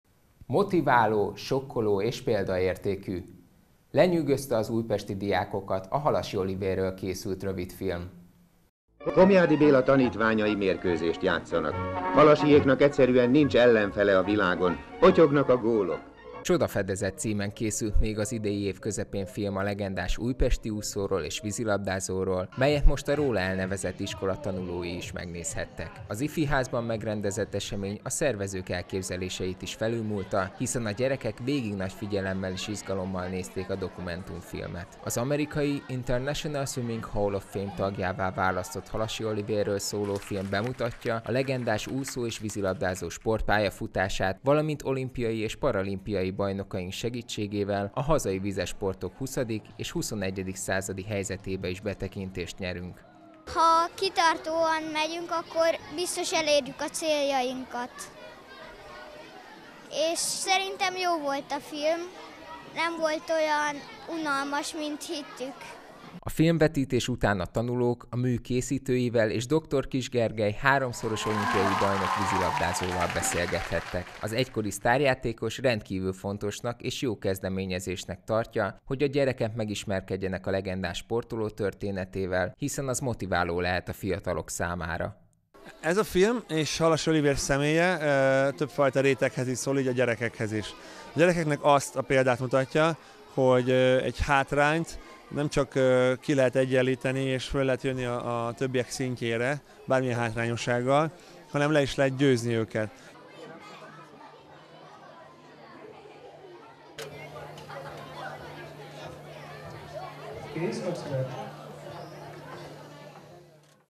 Körkép -5- (2018.11.23.)
Csodafedezet címen készült az idei év közepén film a legendás újpesti úszóról és vízilabdázóról, Halassy Olivérről, melyet most a róla elnevezett iskola tanulói is megnézhettek.